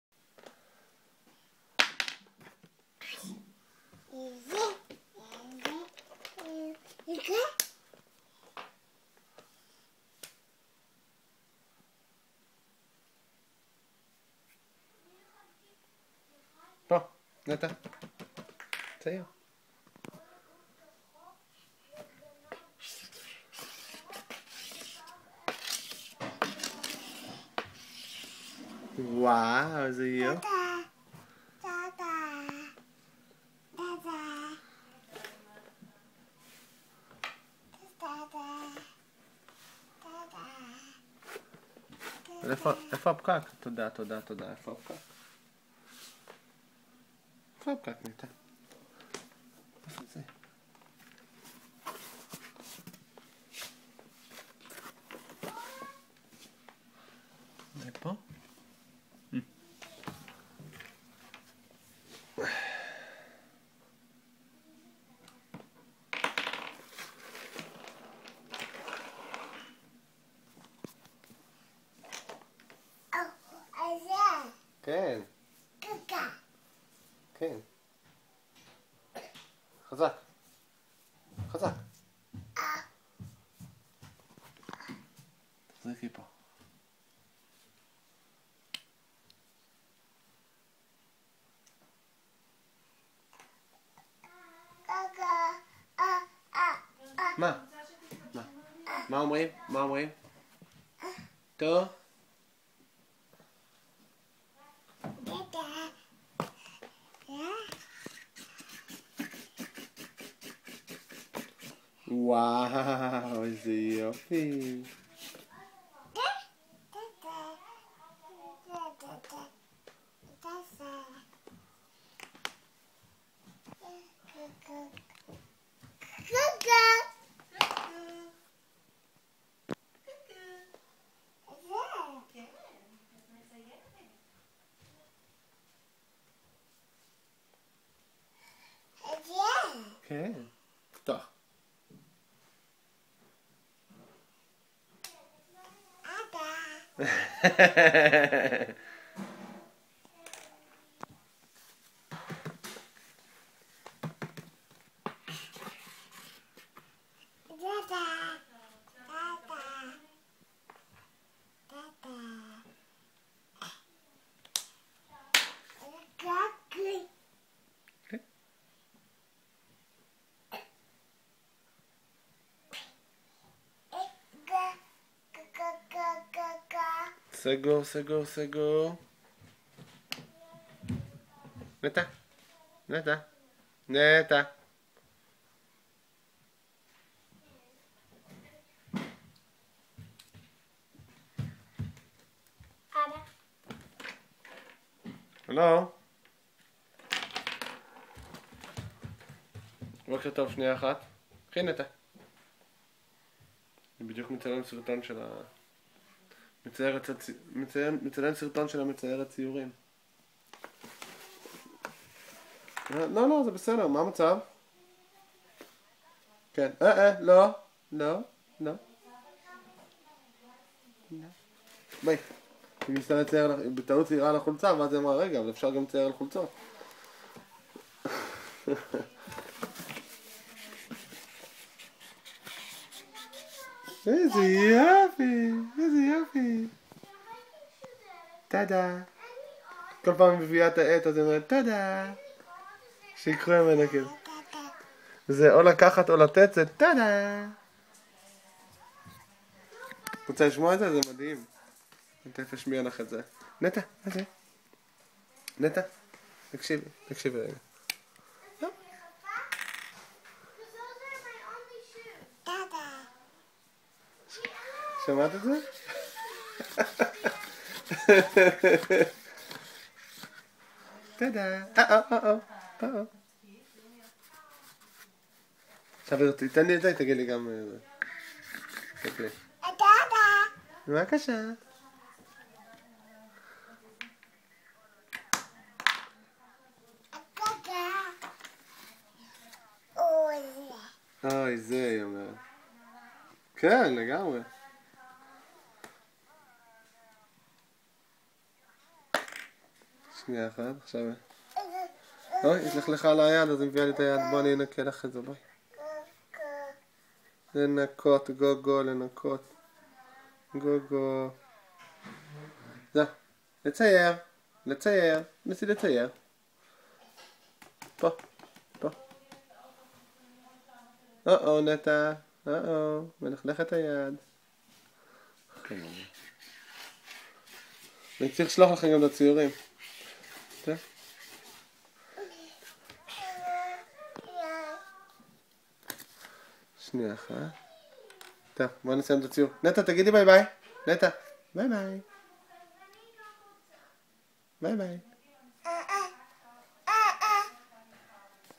Neta Painting
Neta Benayoun painting!  And saying lots of "Thank you" :)